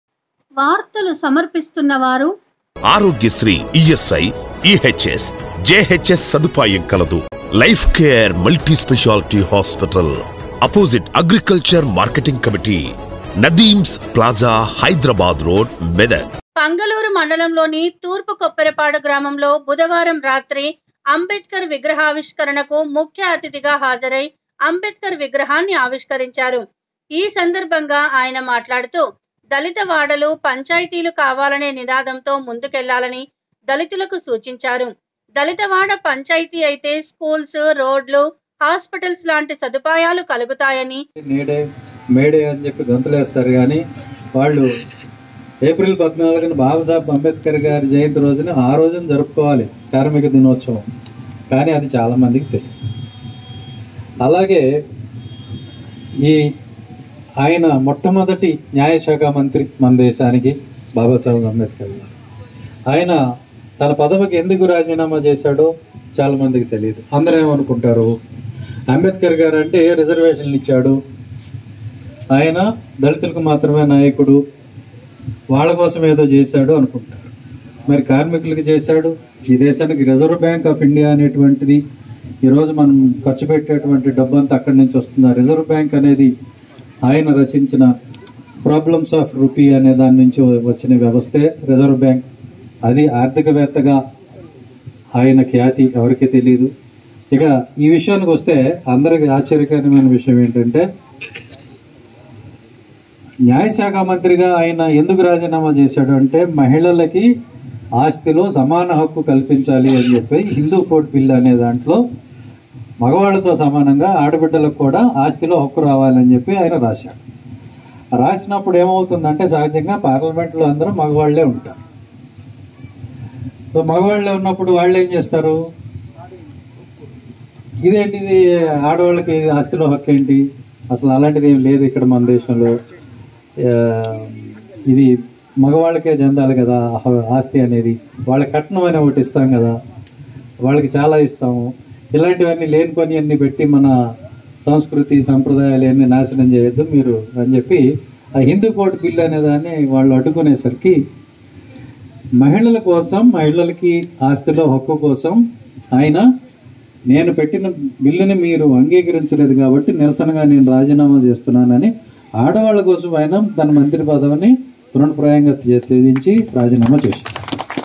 పంగులూరు తూర్పుకొప్పెరపాడులో అంబేద్కర్ విగ్రహాన్ని ఐపీఎస్ అధికారి పీవీ సునీల్ కుమార్ఆవిష్కరించారు
. పంగులూరు మండలంలోని తూర్పు కొప్పెరపాడు గ్రామంలో బుధవారం రాత్రి అంబేద్కర్ విగ్రహ ఆవిష్కరణకు ముఖ్య అతిథిగా హాజరై అంబేద్కర్ విగ్రహాన్ని ఆవిష్కరించారు